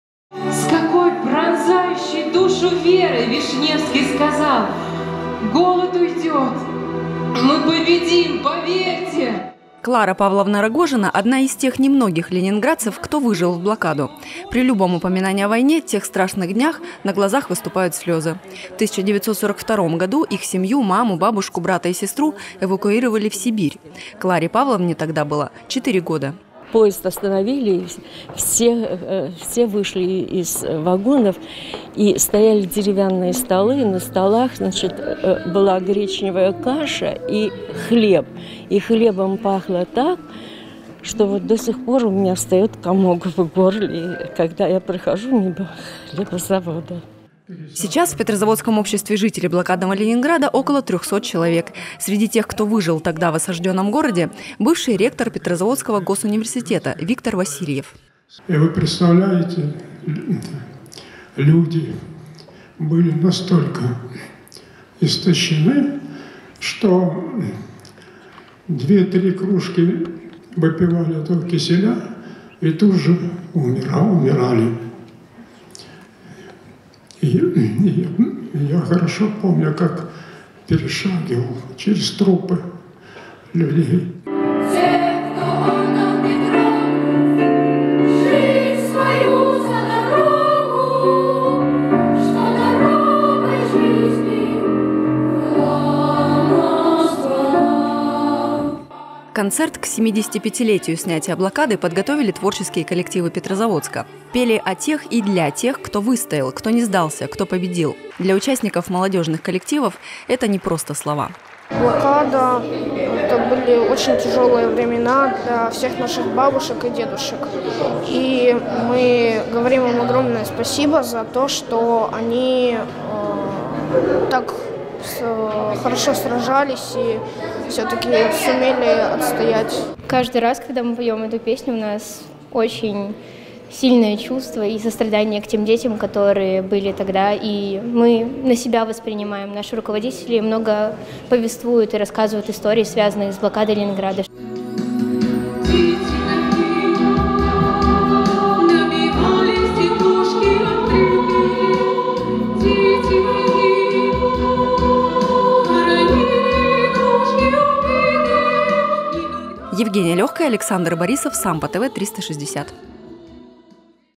Концерт для блокадников
27 января в России отметили 75-летие снятия блокады Ленинграда. От гoлoдa и бoмбёжeк в осаждённом городе погибли, по одним данным, 640 тысяч жителей, по другим данным, не менее одного миллиона человек. В память о тех днях в Петрозаводске состоялась театрально-концертная программа «Мы – дети блокады!». Почётными гостями концерта стали свидетели тех страшных событий - ветераны-блокадники.